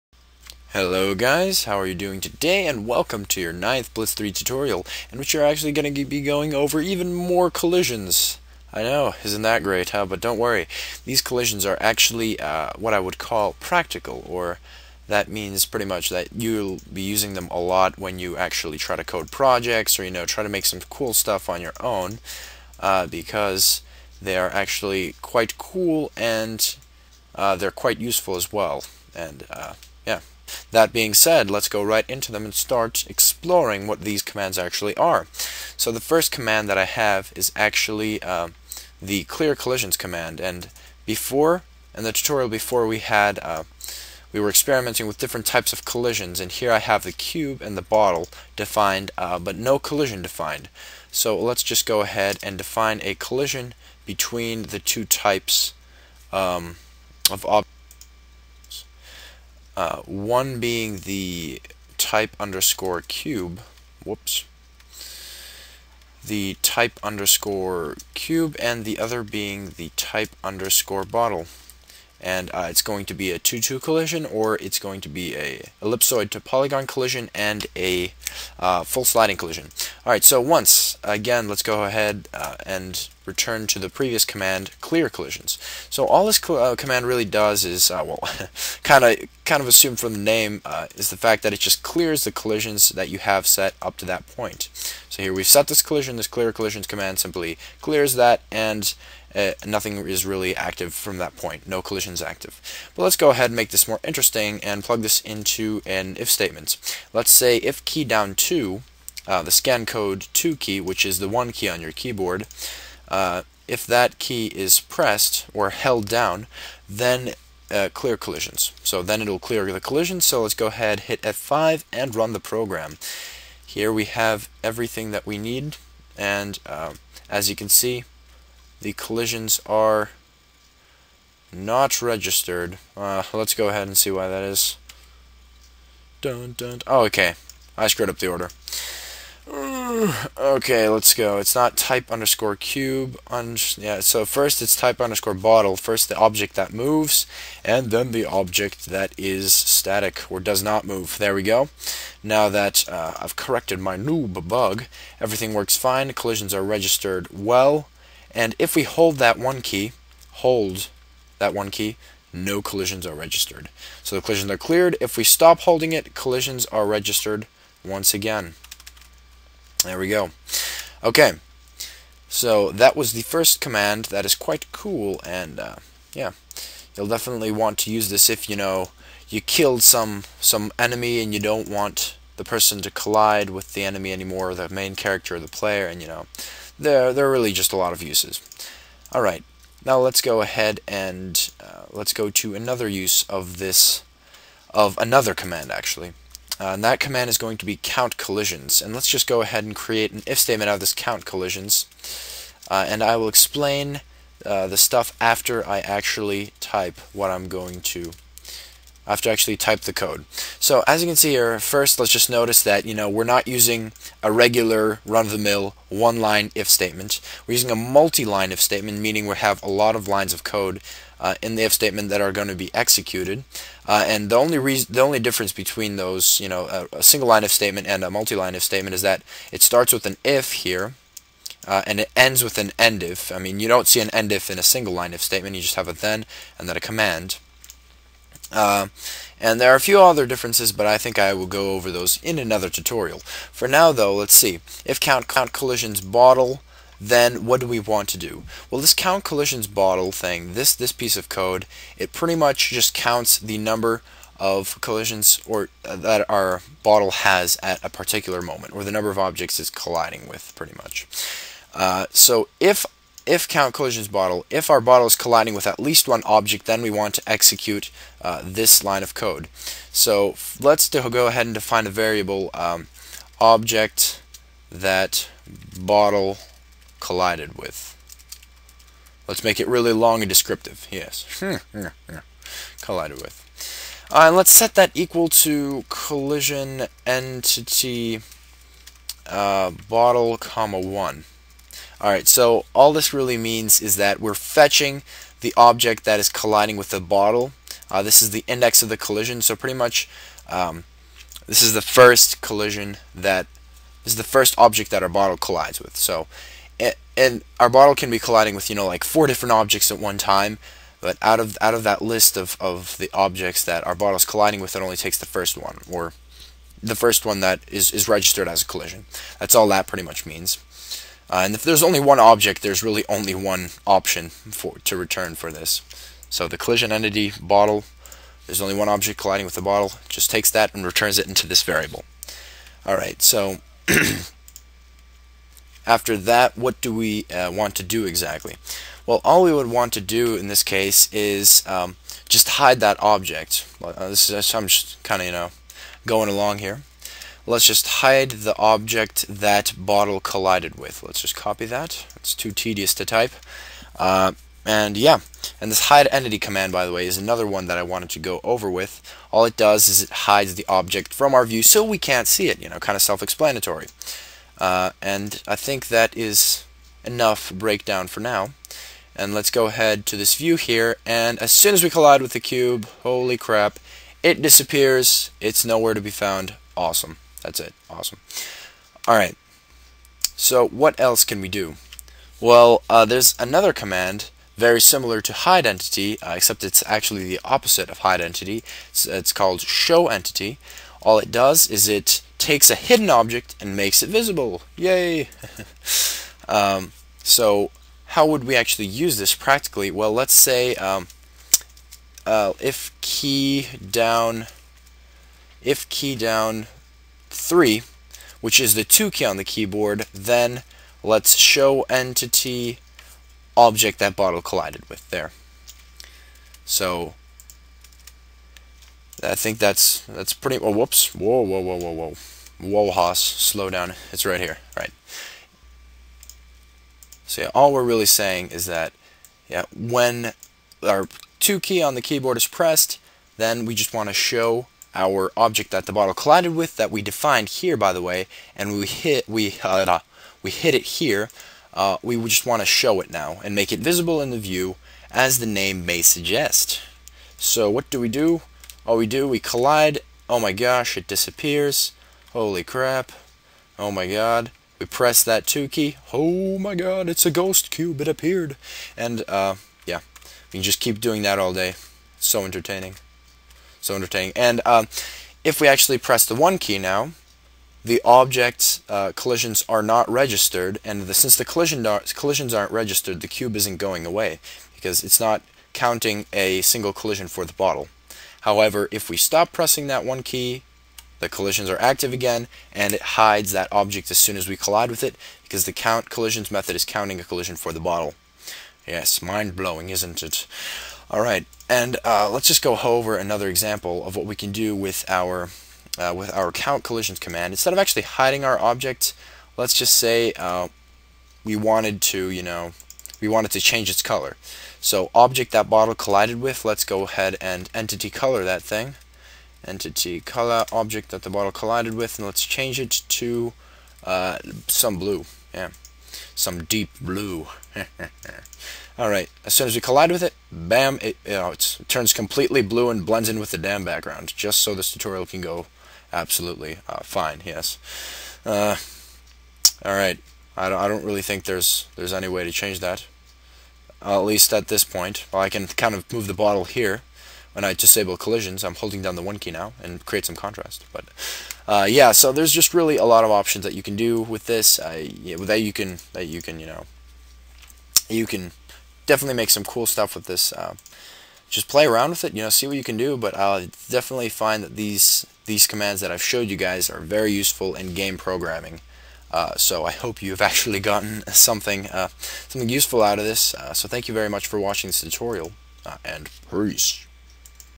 Blitz3D Tutorial - 9 - Uses of Collisions in Game Programming
Yet another video on collisions has been released! I believe this is the last one that will pretty much cover the rest of the stuff we have to know about collisions. In the video, we just take a look at a few different commands, that help us apply collisions to specific aspects of game programming.